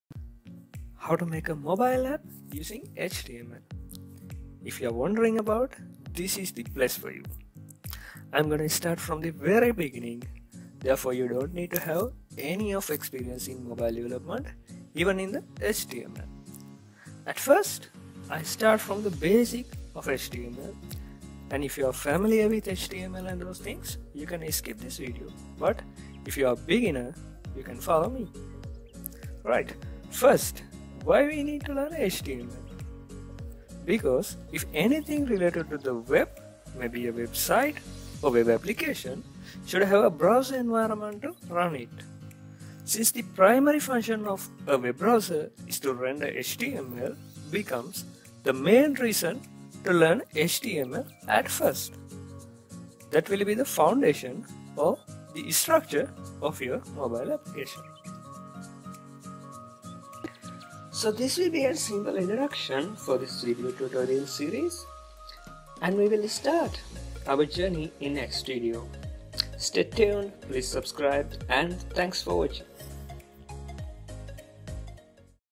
Mobile App Development Using HTML - Introduction - 00
Mobile App Development Using HTML.
if you're wondering about, this is the place for you.
This is an simple Introduction for this video series.
I will guide you from the very beginning of HTML up to Mobile application in step by step.

And also if you are searching for HTML for beginners tutorial. this series will obviously what you're searching for because I'll do all the HTML, CSS , JavaScripts, and all up to Mobile development therefore you can stop at any where you want when you grab what you need. so please subscribe and stay with The Awesome.